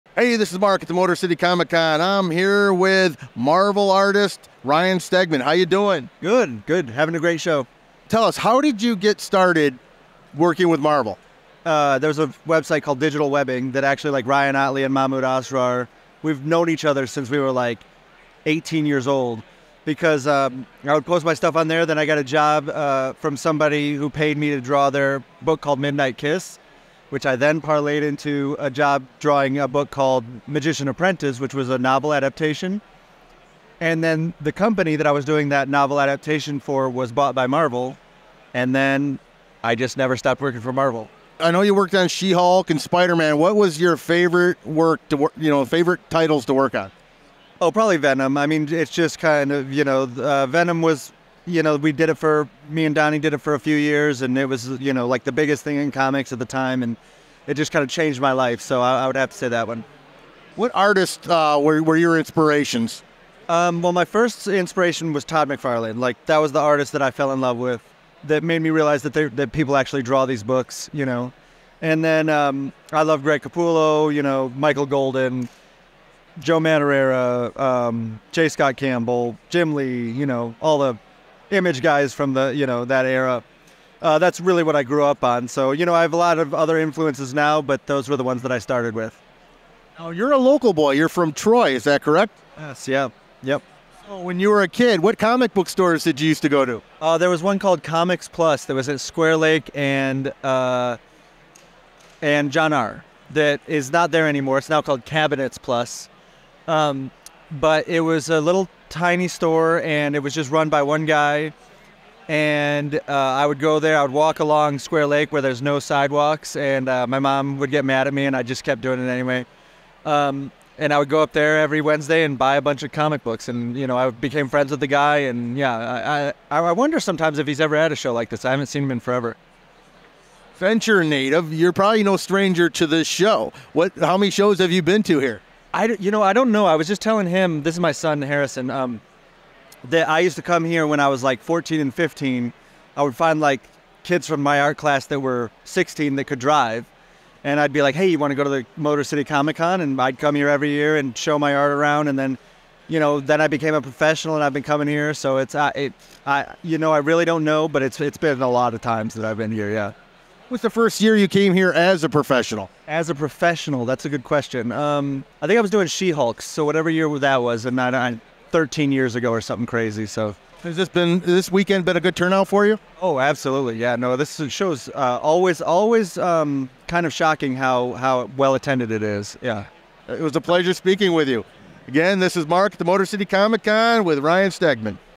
Ryan Stegman
The incredible comic book artist Ryan Stegman shared how he got his start with Marvel, working on Venom, his influences, collecting comics in his hometown of Troy, Michigan and attending the Motor City Comic Con in high school and as a professional artist.